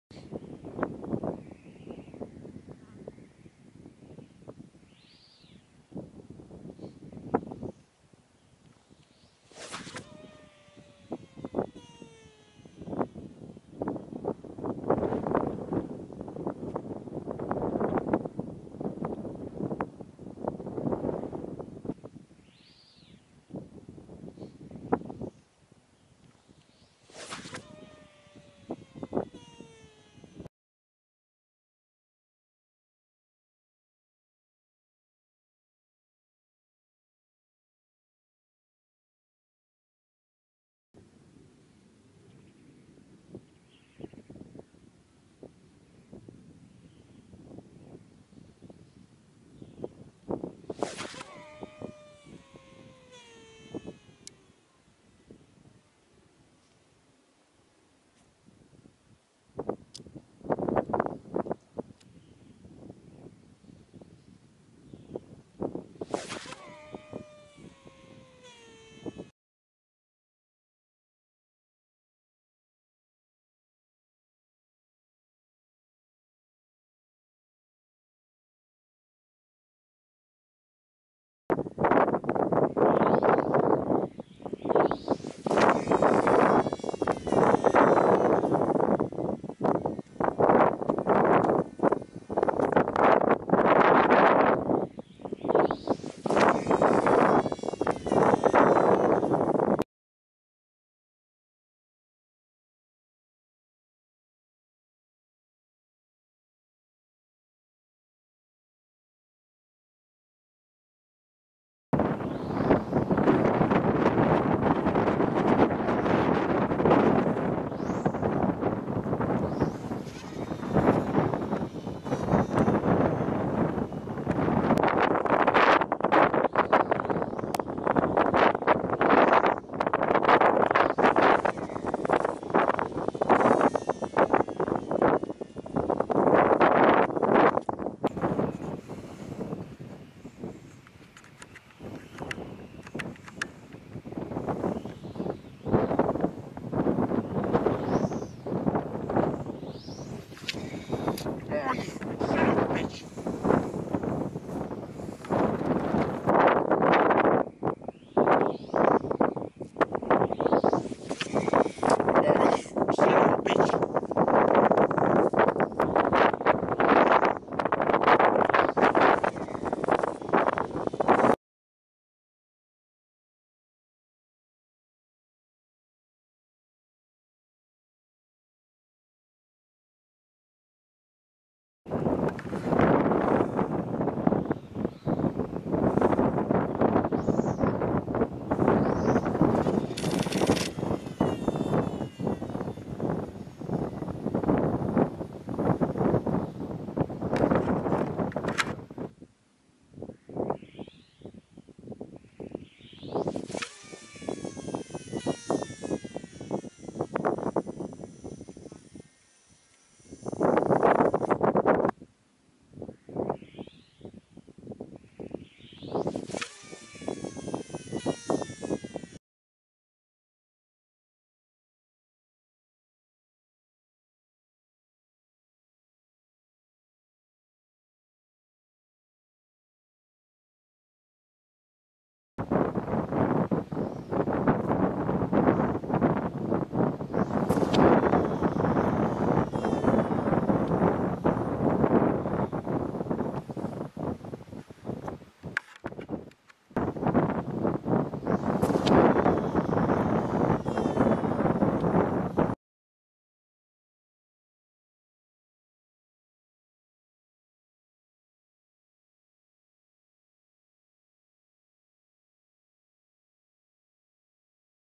Century and 125 gram
Finally got up to Lancefield and was able to measure the casts . Average was in the 240 meter range with a best of 247.9 meters . Very happy with that .